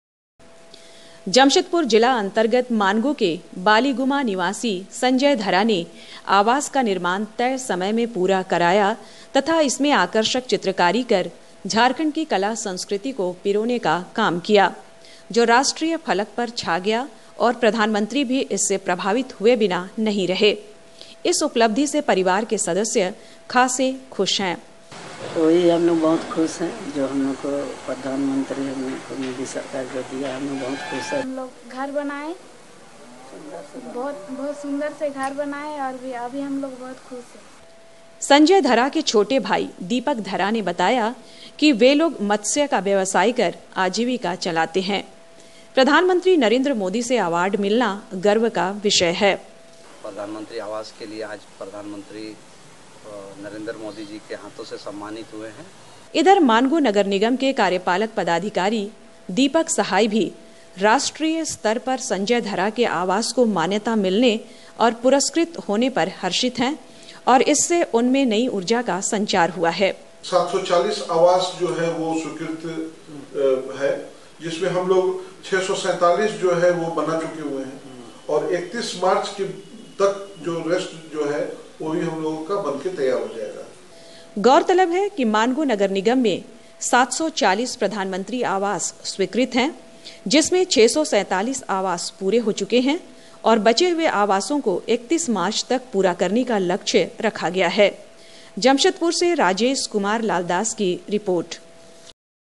बेहरतरीन आवास के लिए सम्मान
जमशेदपुर जिला अंतर्गत मानगो के बालीगुमा में बने संजय धरा के प्रधानमंत्री आवास को झारखंड राज्य में उत्कृष्टता का पहला पुरस्कार मिला है। प्रधानमंत्री नरेन्द्र मोदी ने दिल्ली में आयोजित ऑनलाइन कार्यक्रम में संजय धरा को पुरस्कृत किया।